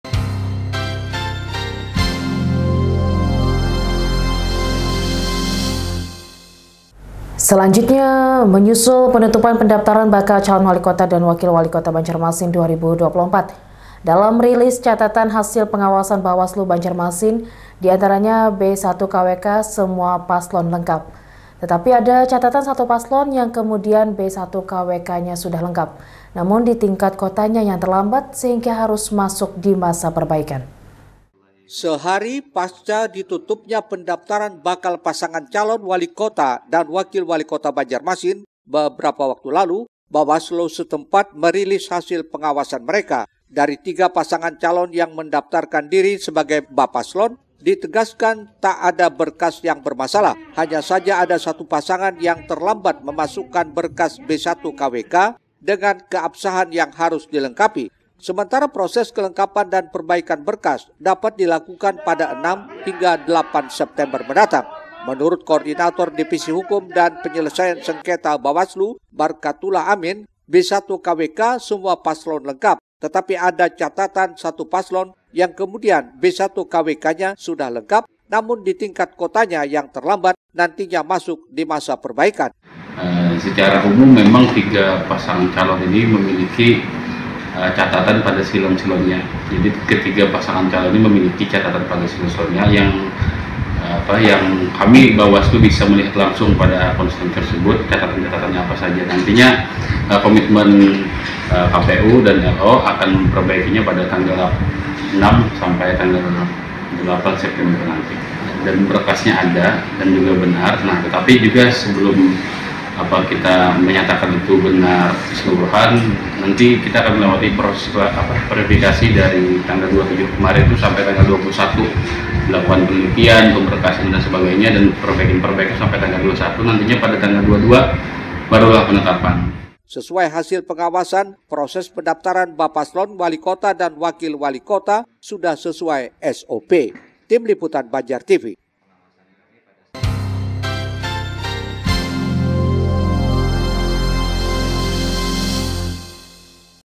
BAWASLU PASTIKAN BERKAS B1KWK PASLON PILWALI AMAN
Menyusul penutupan pendaftaran Bakal Calon Walikota dna Wakil Walikota Banjarmasin 2024, dalam rilis catatan hasil pengawasan BAWASLU Banjarmasin, diantaranya b1kwk semua paslon lengkap, tetapi ada catatan satu paslon yang kemudian b1kwknya sudah lengkap, namun di tingkat kotanya yang terlambat sehingga harus masuk dimasa perbaikan.